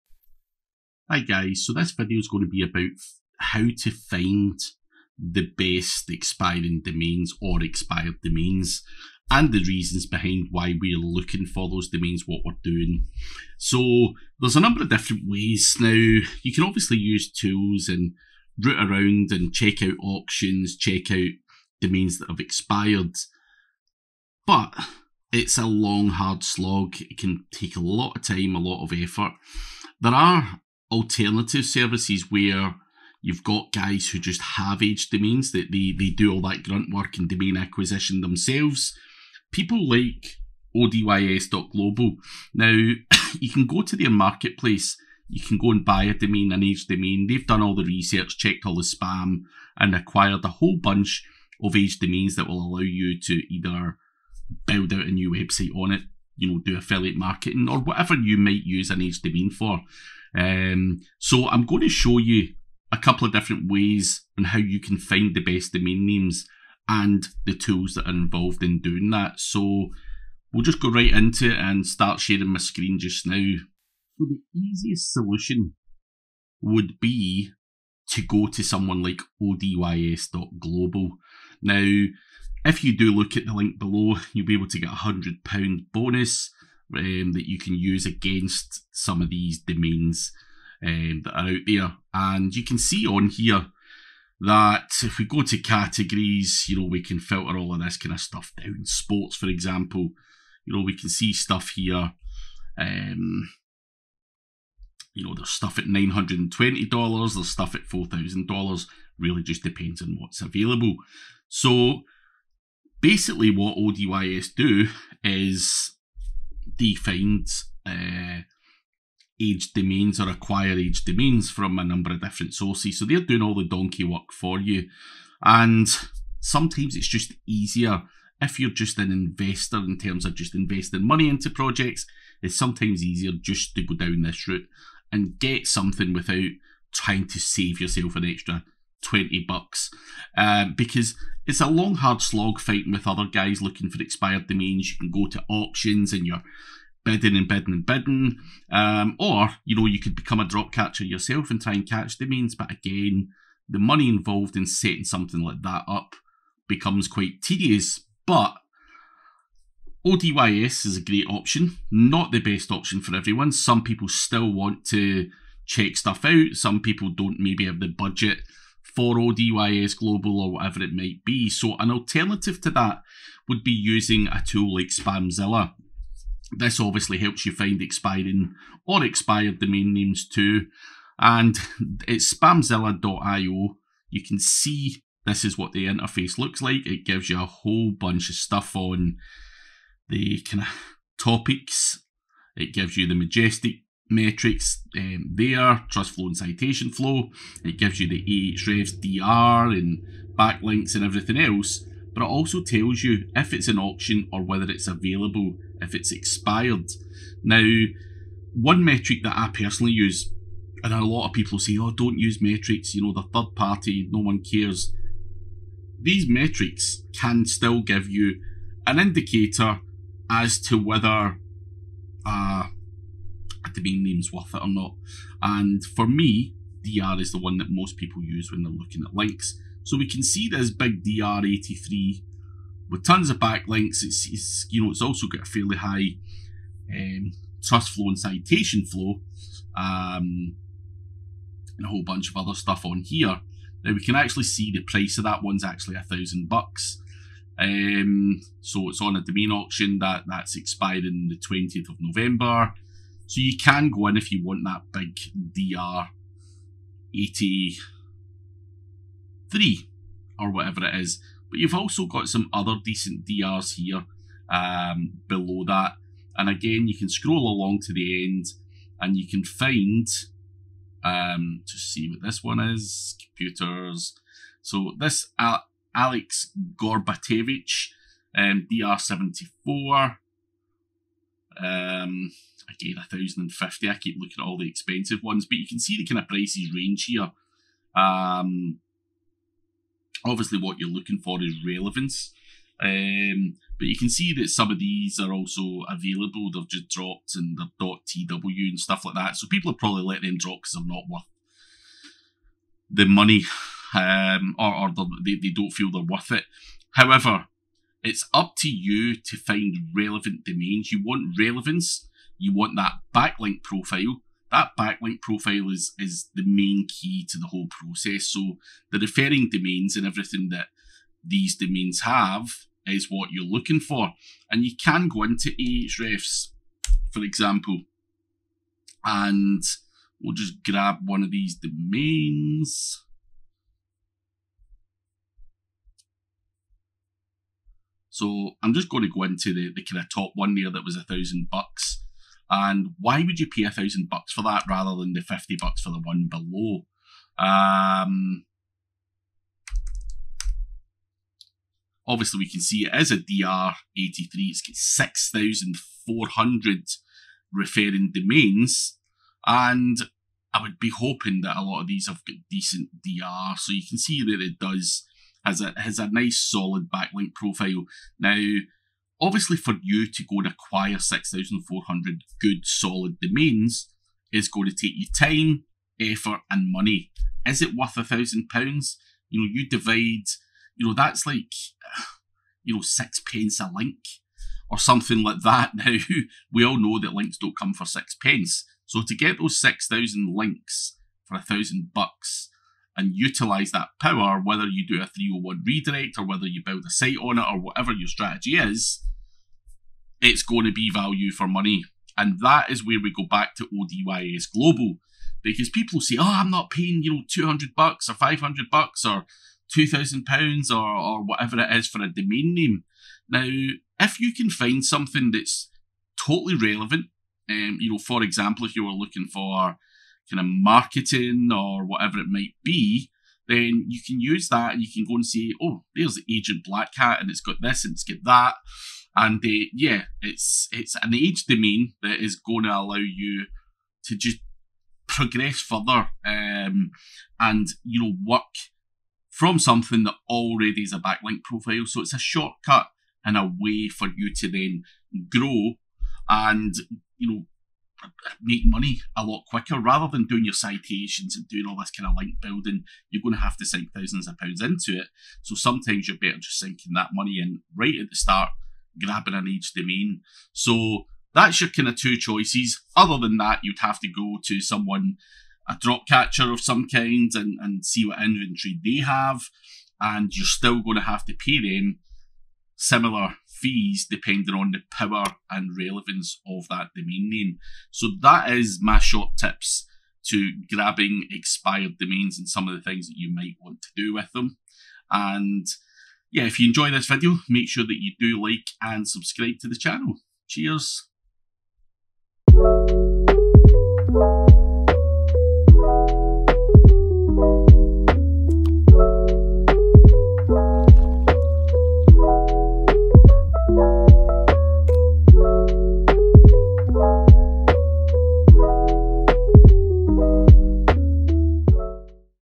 How to find Good Expired Domains, Find Aged Domains
This video shows you how you can go about finding good quality expired domains, these domains can help kickstart your affiliate journey or give you a decent backlink profile that you can simply redirect to a new domain.